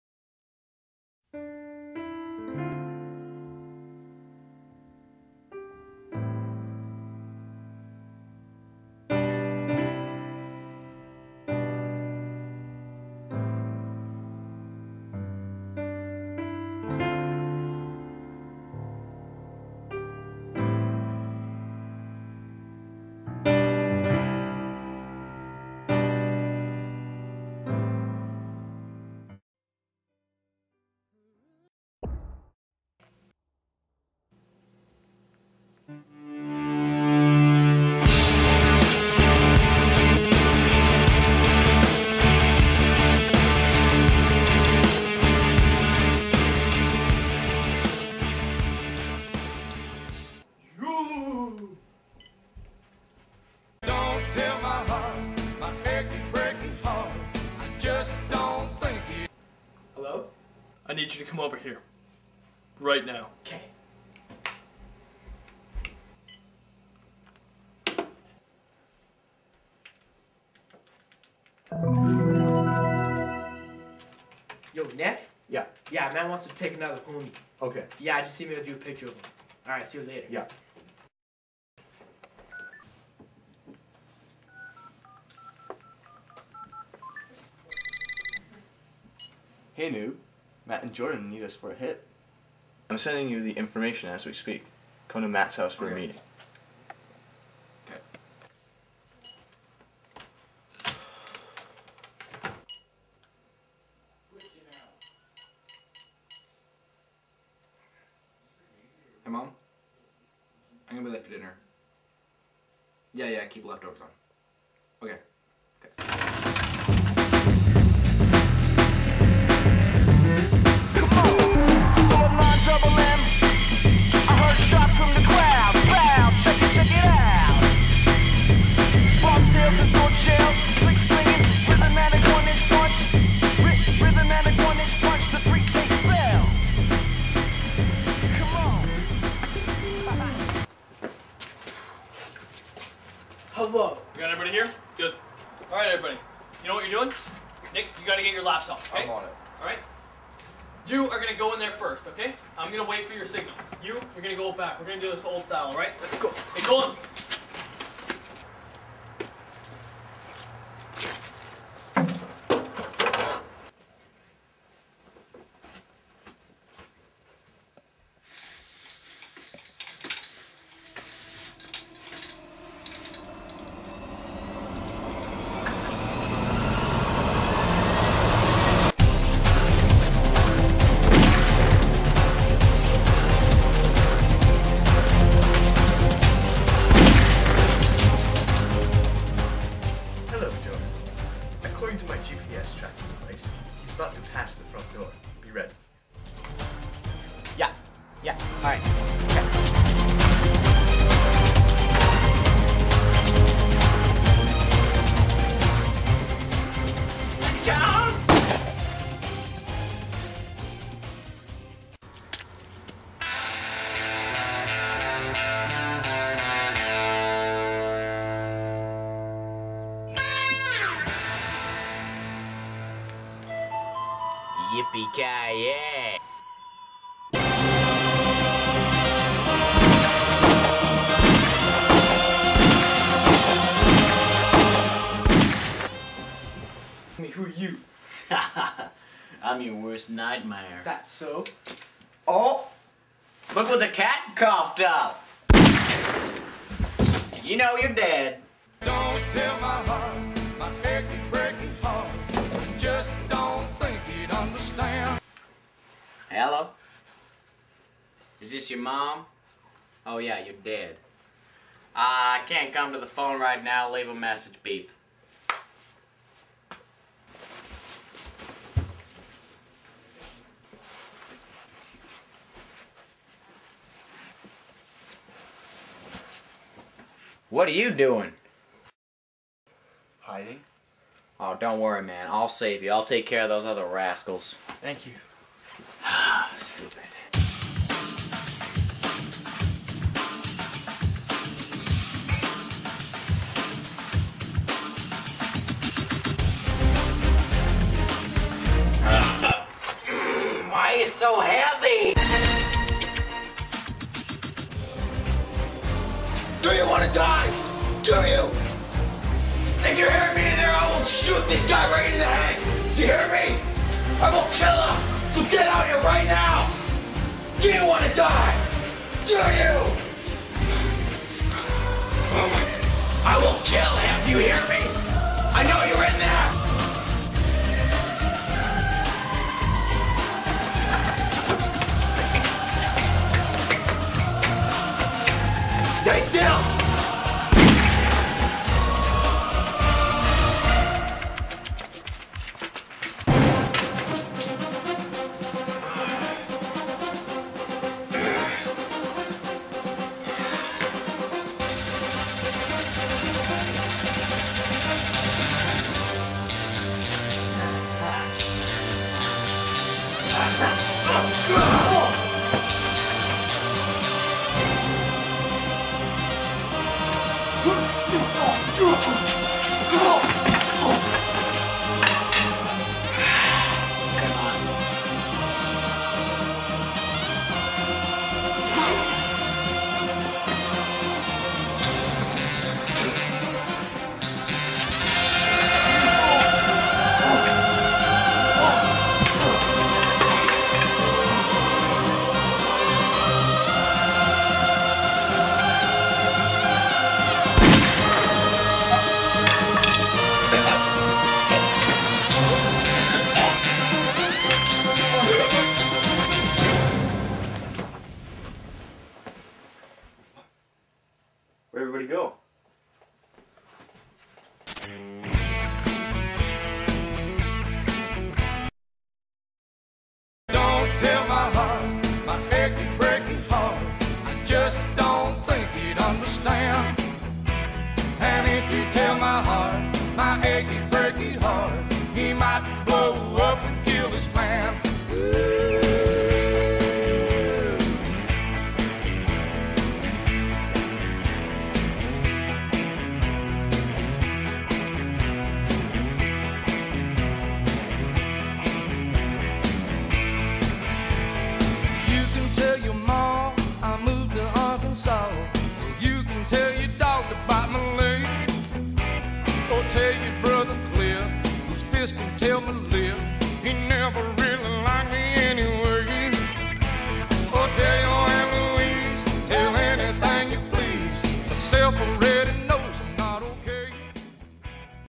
The Friend We Hate
The Friend We Hate is a remake of an earlier pokerchip productiosn film ,It is about 4 friends trying to take out a friend that they all hate. An amazing twist keeps them all trapped in an Action movie enviroment!